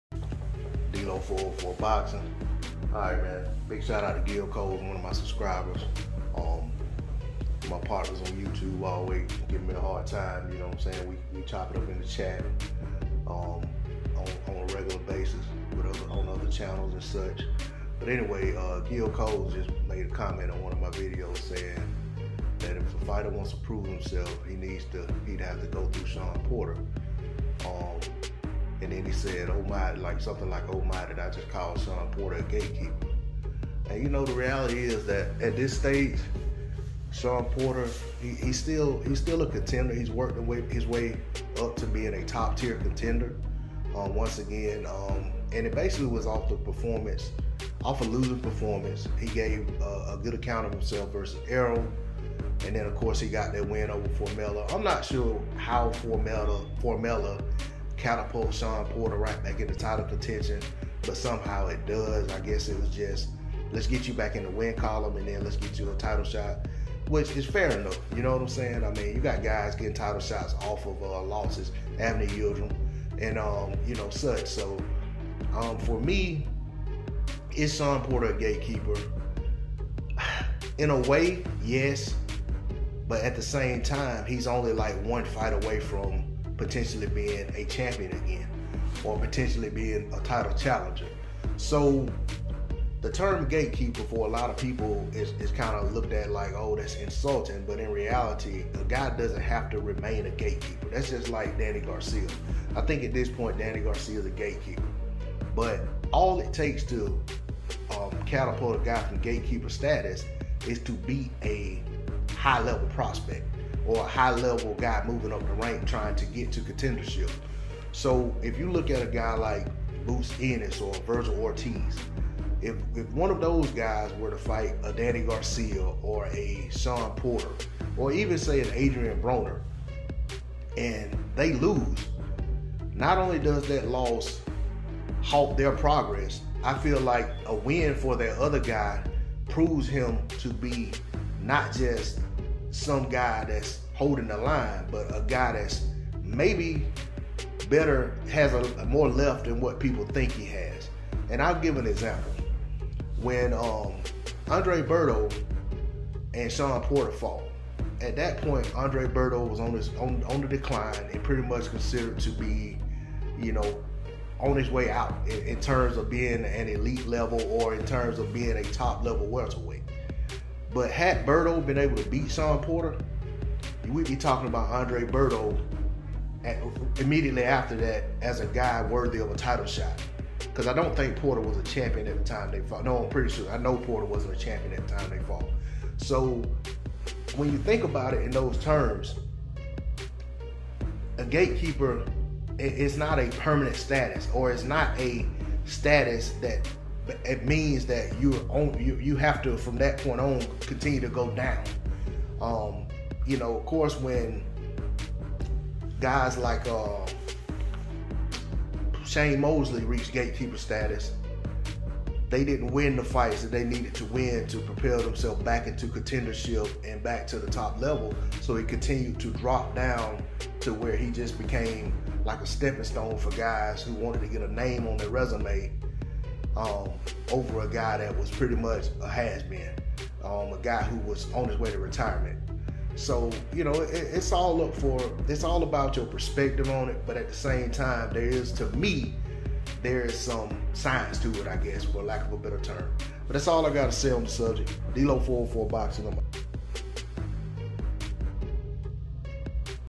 Are Shawn Porter & Danny Garcia Gatekeepers In 2021
my thoughts on it and more about gatekeepers   etc